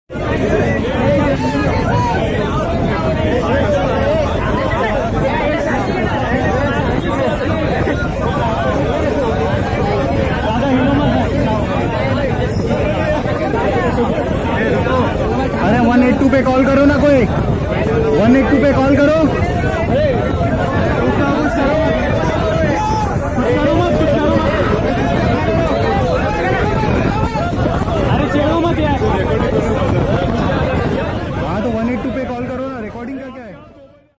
🐍🚆 Mumbai Local Train Mein Mila Saap (Snake)
🐍 Titwala aur CST Ke Beech Train Mein Mila Saap

⏱ Subha 8:39AM Ki Local Train Mein Tha Snake

🐍🚃 Local Train Ke Fan Par Green-Vine Snake Dikahyi Diya Yatrion Me Machi khalbali Train kuch Der Roki Gayi Thane Station

📲 Mumbai Local News

 🔔 Bell Icon Dabana Na Bhule To Get Regular